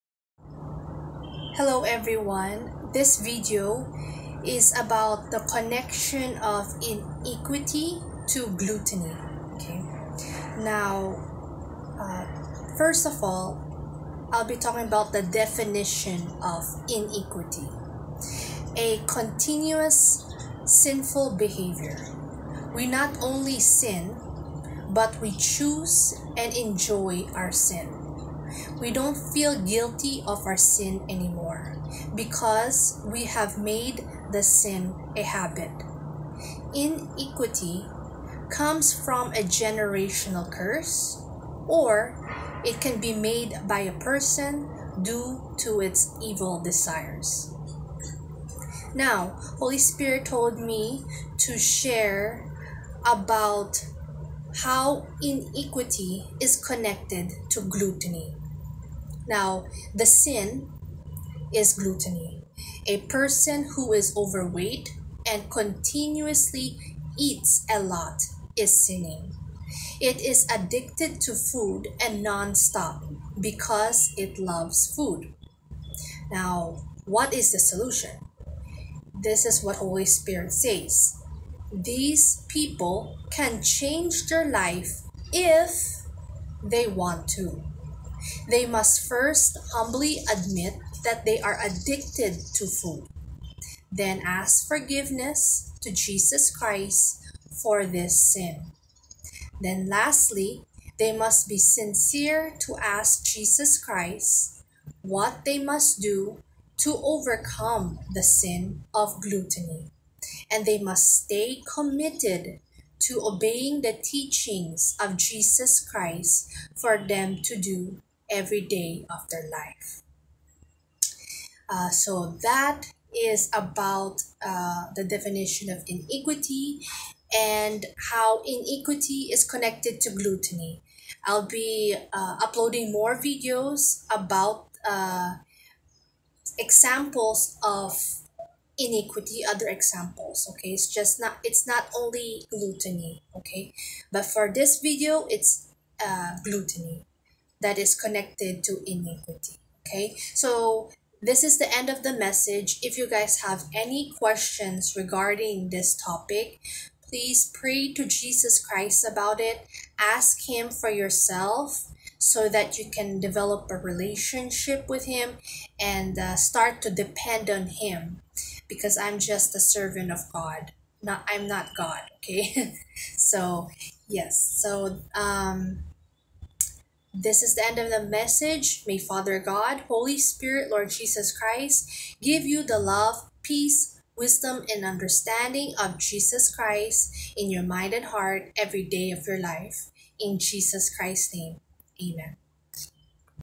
Connection of iniquity to gluttony
Definition of iniquity:  a continuous sinful behavior. We not only sin, but we choose and enjoy our sin. We don't feel guilty of our sin anymore, because we have made the sin a habit. Iniquity comes from a generational curse or made by a person due to its evil desires.

Holy Spirit says:  This is how iniquity is connected to gluttony.

Sin:  A person who is overweight and continuously eats a lot is sinning.  It is addicted to food and non-stop because it loves food.

Solution: These people can change their life IF they want to.  They must first humbly admit that they are addicted to food, then ask forgiveness to Jesus Christ for this sin.  Lastly, they must be sincere to ask Jesus Christ what they must do to overcome the sin of gluttony and they must stay committed to obeying the teachings of Jesus Christ for them to do daily.